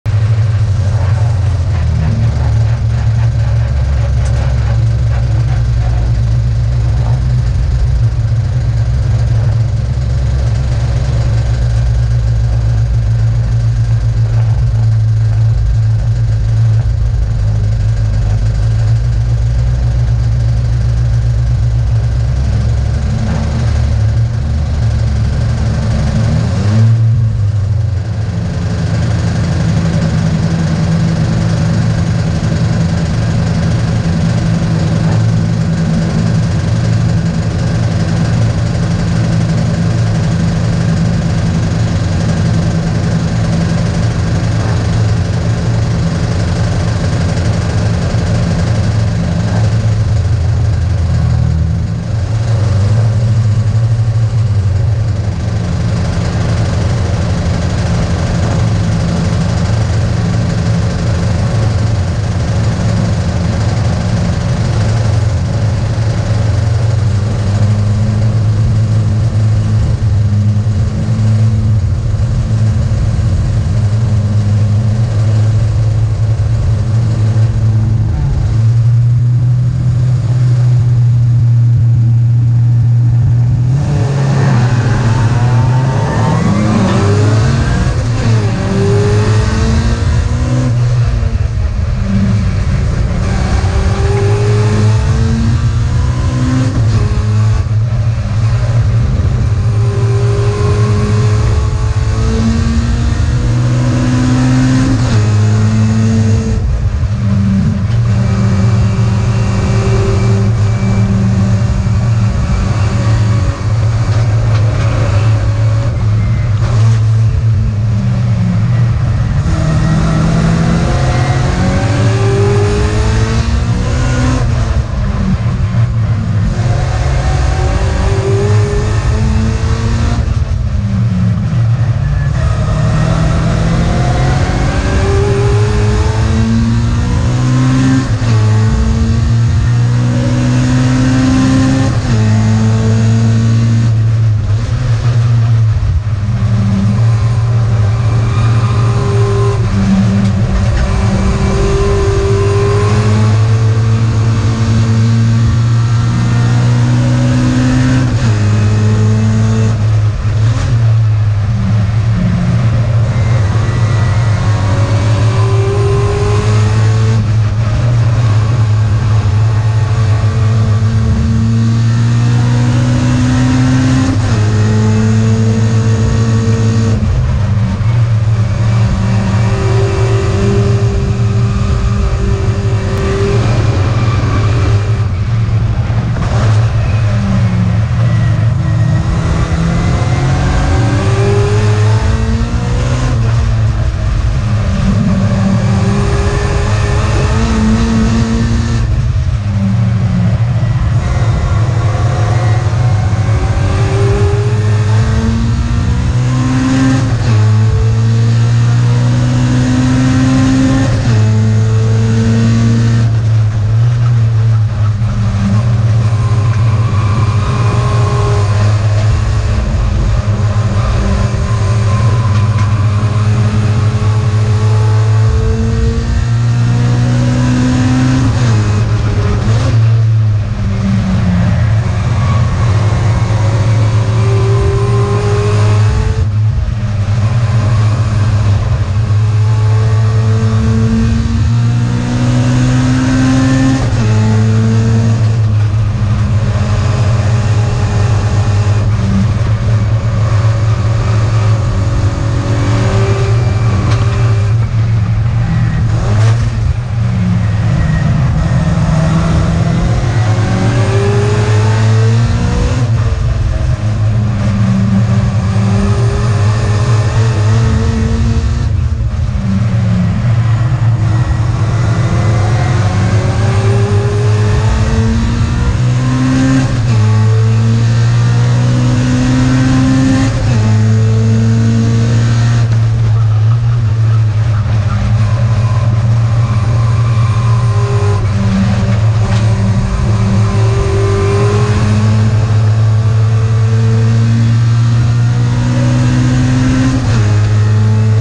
Race 3 Historic Winton 2022 in the SU Powered Nb Mini Cooper S
Watch the first few laps of Race 3 in the mighty SU Powered Nb 1964 Mini Cooper 'S, driven by Adam Bressington.